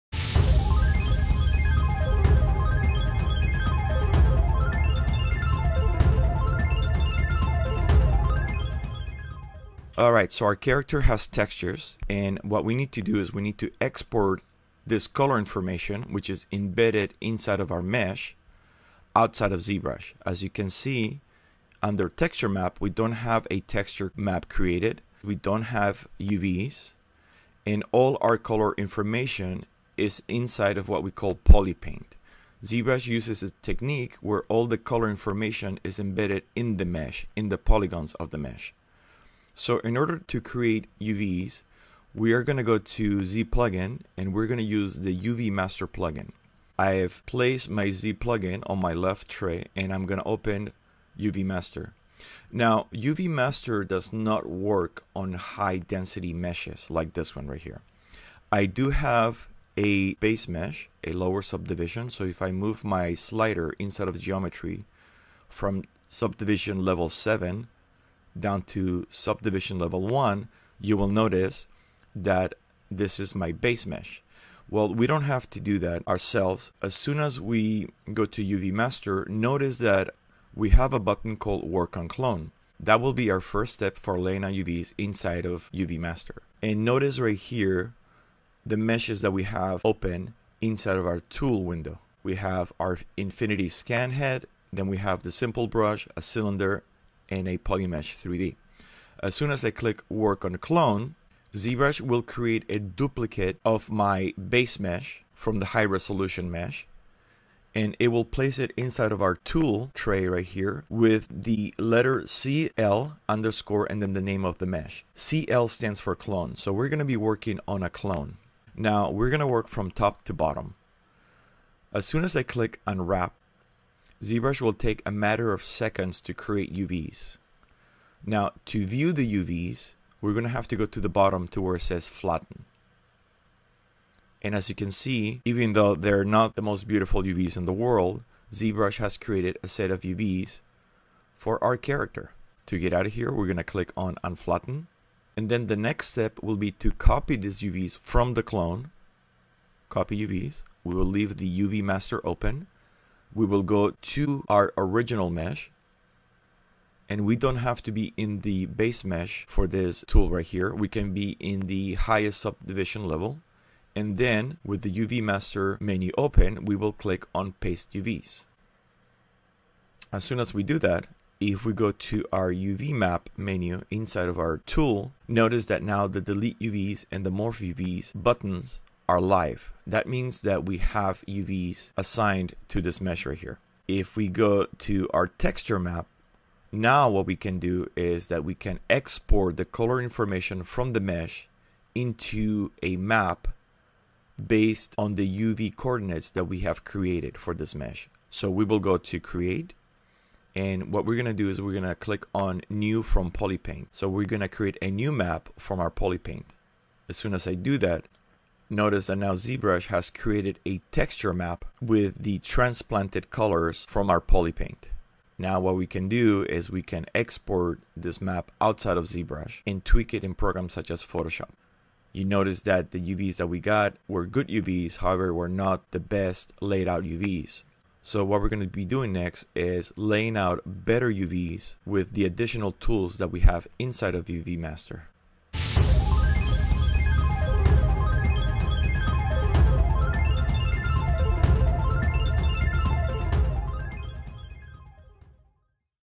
DART 153 Intro To ZBrush: UVs Part 1 Basic UV Creation
Learn how to quickly create UVs for your Tool in ZBrush using UV Master. We will learn the basic creation of a Clone, how to Unwrap and how to copy and paste the UVs from the Clone onto the original, hi-res mesh...

For more information on this class, DART 153 F - Introduction To Digital Sculpting with ZBrush, please